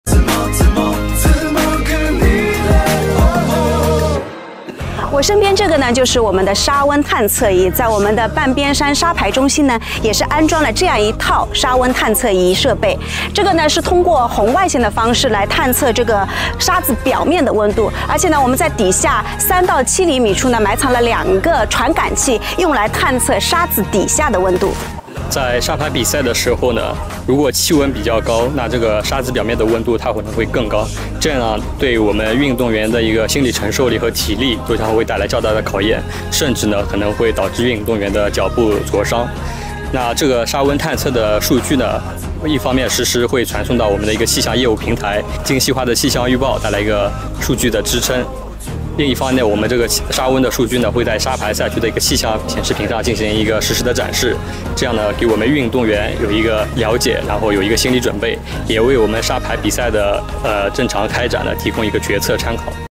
The Asian Games beach volleyball base on Banbian Mountain
One of the key focuses is monitoring the sand temperature for the beach volleyball competition. The meteorological support team has set up two sand temperature observation stations at the Asian Games beach volleyball base on Banbian Mountain and at the County Meteorological Bureau.
These stations utilize sand temperature sensors to monitor real-time surface and subsurface temperatures of the sand. Let's take a closer look!